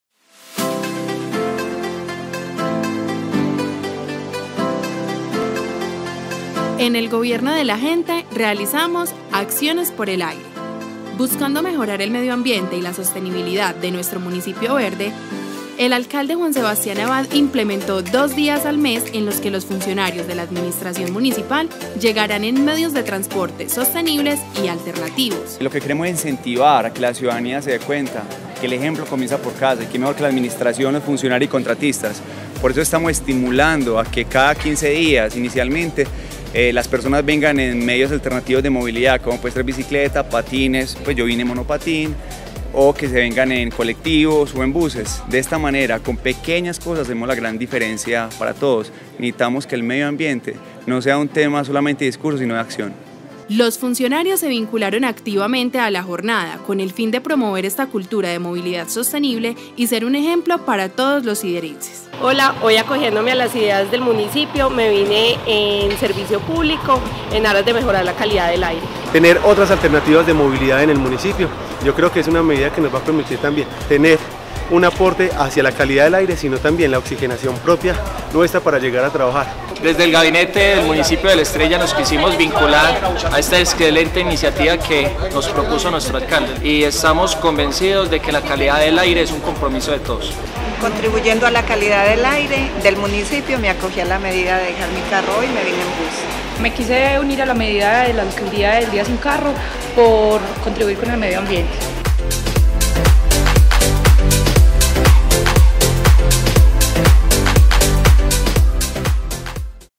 Alcaldía sin Carro
Con el fin de promover AccionesPorElAire para la conservación de un ambiente sano, nuestro Alcalde Juan Sebastián Abad, junto a sus funcionarios llevaron a cabo la medida administrativa de Alcaldía sin Carro, a través de la movilidad activa y sostenible para seguir haciendo de nuestro municipio el mejor lugar para la vida.